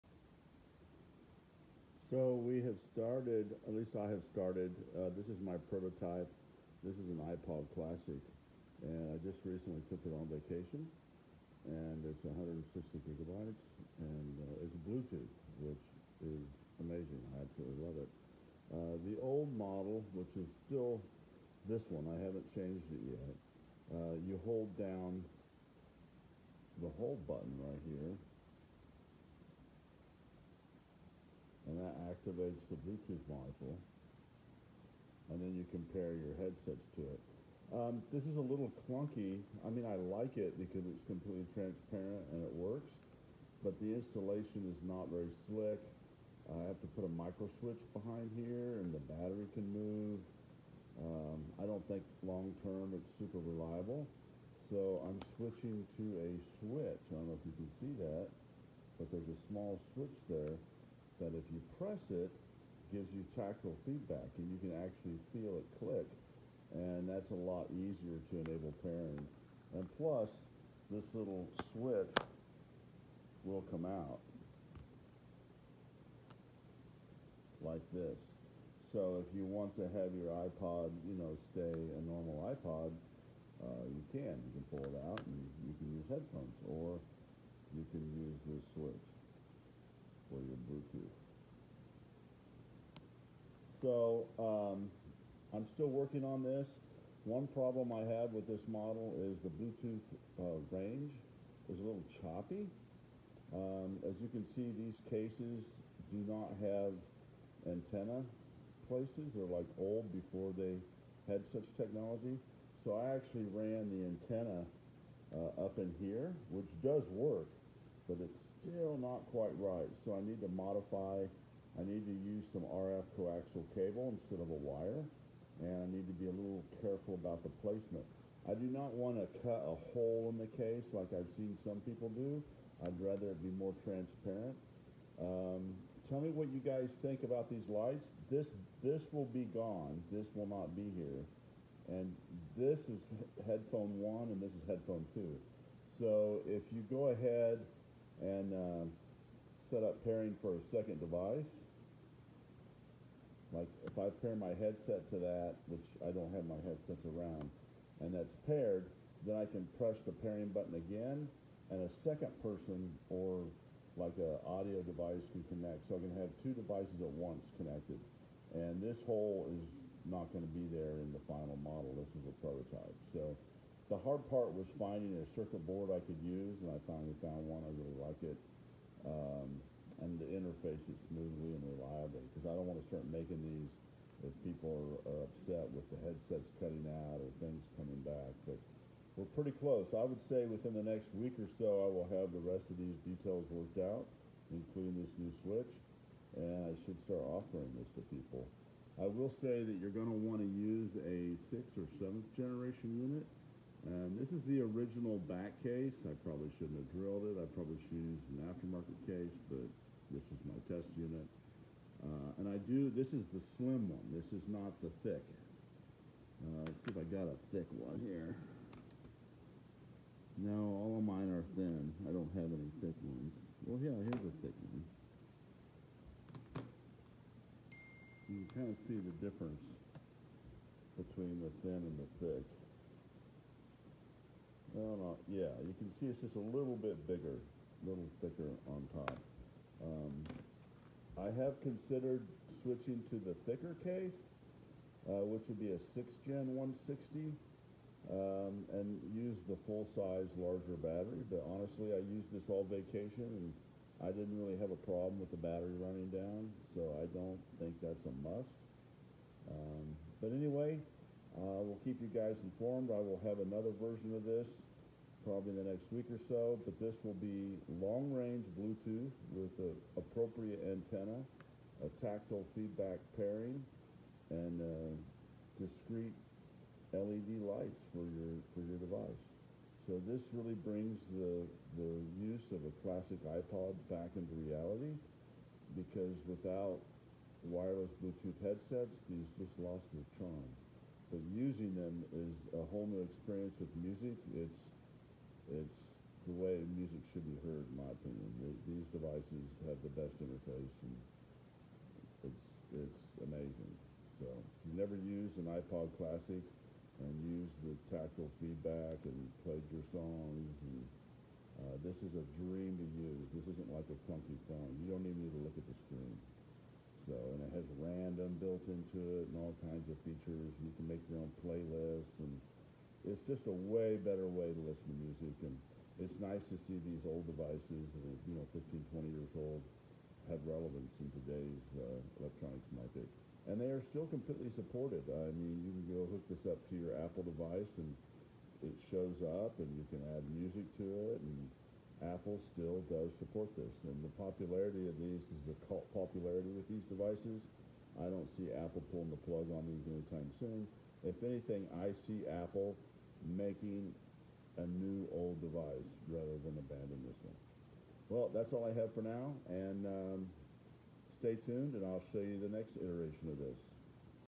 Ipod Classic Mod. Bluetooth. Supports two bluetooth devices at the same time!
iPod classic with Bluetooth wireless interface!

This is my first prototype.   I just used it on vacation and loved it!   I was on a cruise and Cell signal or WiFi was not a good option.  I plan to make a few changes before I am ready to release to the public.

Future changes will be the tactile click paring button and an upgraded bluetooth antenna.  Current range is about 15 feet.  I expect 75 feet with upgraded antenna.   The unit currently has a 256GB solid state storage " more than needed to index 60,000 mp3 files".  60,000 is the maximum database index so more than a 256GB chip is a waste.

The final product will have the click pairing button and will have 512 GB storage.   Will only have two indicator lights for pairing with two devices.  It will also feature a long range bluetooth antenna instead of the shorter 15 foot range.

I hope to offer this as a service and will be selling these units within the next month depending on how testing goes.